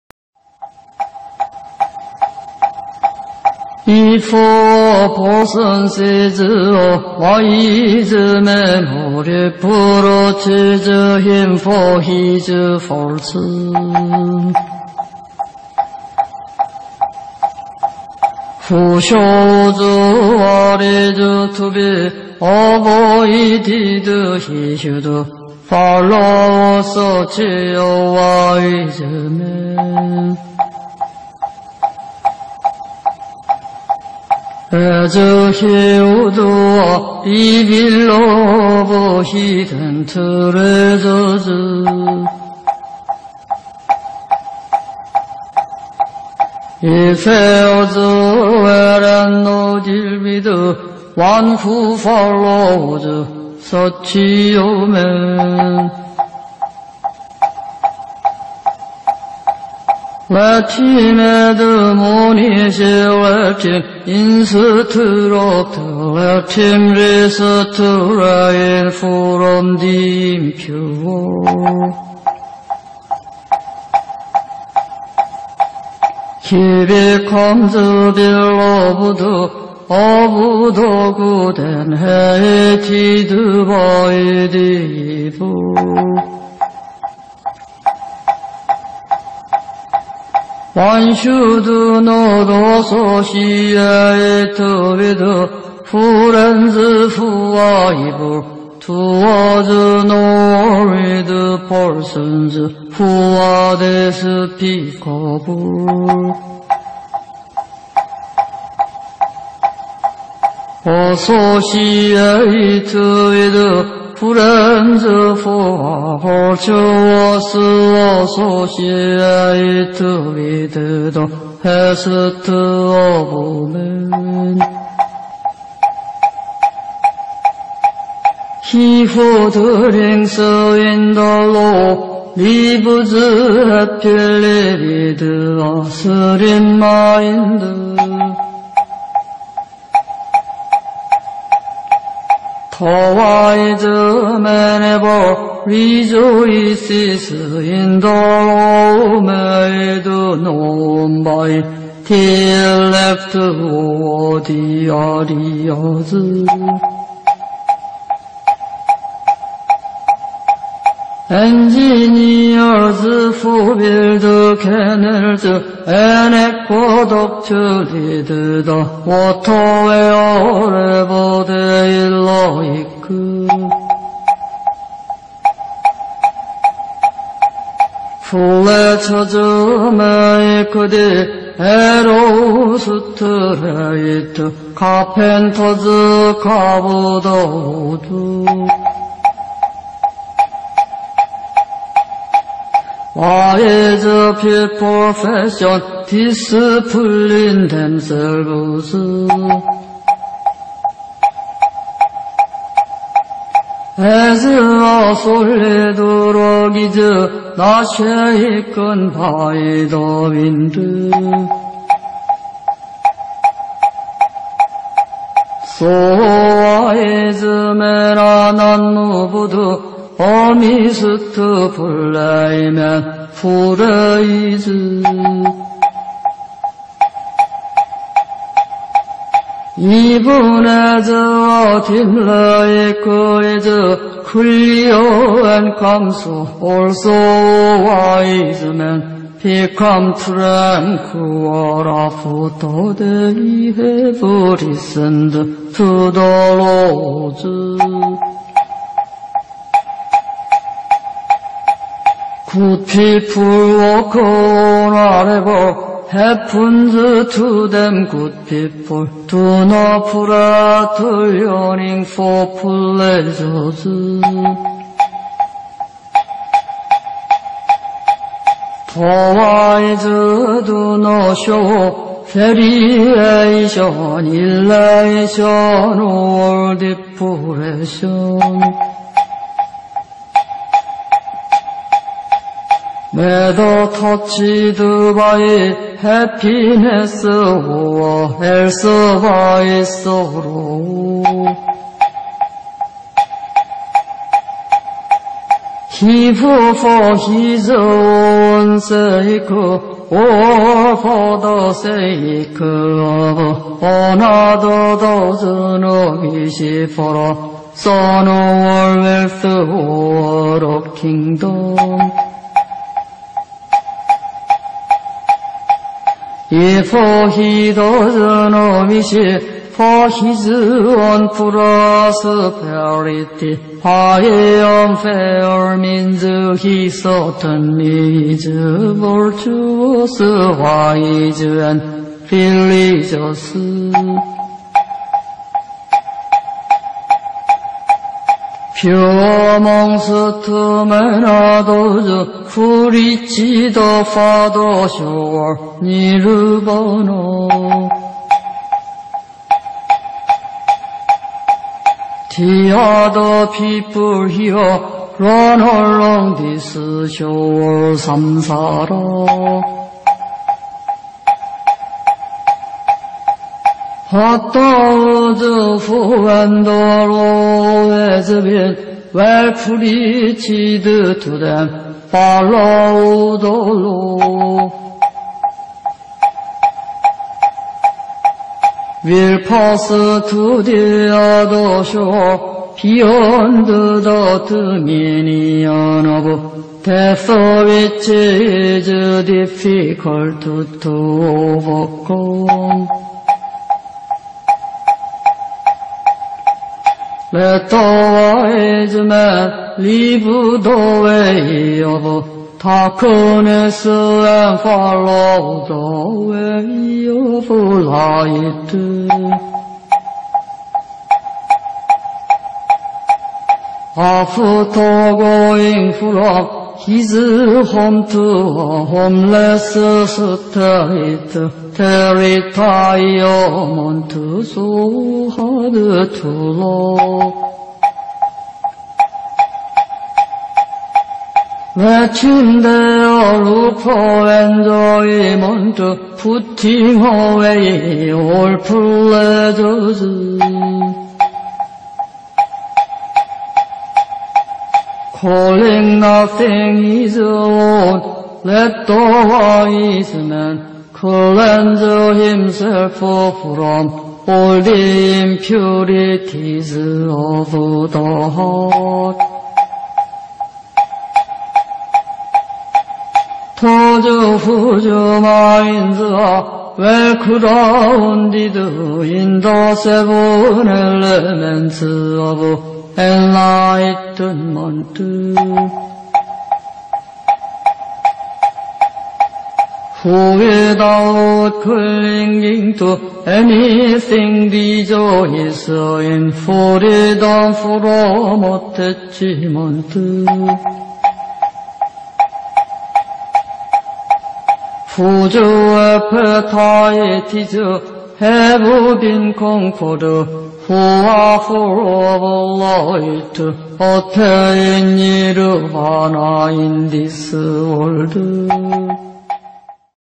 영어법구경 제6장[76句부터~89句까지] 보우스님독송
- 第 6 章. 현철품(賢哲品) - 현자(賢者)의 장 THE WISE MAN 6, 지혜로운 사람
76

If a person sees a wise man who reproaches him (for his faults), who shows what is to be avoided, he should follow such a wise man as he would a revealer of hidden treasures. It fares well and is not ill with one who follows such a man.

77

Let him admonish, let him instruct, let him restrain from the impure. He becomes beloved of the good and hated by the evil.

78

One should not associate with friends who are evil-doers nor with persons who are despicable; associate with friends who are virtuous, associate with the best of men.

79

He who drinks in the law lives happily with a serene mind. The wise man ever rejoices in the law made known by the elect (or the Aryas).

80

Engineers (who build canals and aqueducts) lead the water (wherever they like), fletchers make the arrow straight, carpenters carve the wood; wise people fashion (discipline) themselves.

81

As a solid rock is not shaken by the wind, so wise men are not moved amidst blame and praise.

82

Even as a deep lake is clear and calm so also wise men become tranquil after they have listened to the laws.

83

Good people walk on whatever happens to them. Good people do not prattle, yearning for pleasures. The wise do not show variation (elation or depression), whether touched by happiness or else by sorrow.

84

He who, for his own sake or for the sake of another, does not wish for a son or wealth or a kingdom, if he does not wish for his own prosperity by unfair means he certainly is virtuous, wise, and religious.

85

Few amongst men are those who reach the farther shore*: the other people here run along (this) shore. [Note: The other shore (farther shore) stands for life eternal, nirvana; this shore for earthly life, samsara.]

86

But those who, when the law has been well preached to them, follow the law, will pass to the other shore, [beyond] the dominion of death which is difficult to overcome.

87

Let the wise man leave the way of darkness and follow the way of light. After going from his home to a homeless state, that retirement is so hard to love.

88

Let him there look for enjoyment. Putting away all pleasures, calling nothing his own, let the wise man cleanse himself from all the impurities of the heart.

89

Those whose minds are well grounded in the (seven) elements of enlightenment, who without clinging to anything rejoice in freedom from attachment, whose appetites have been conquered, who are full of light, attain nirvana in this world.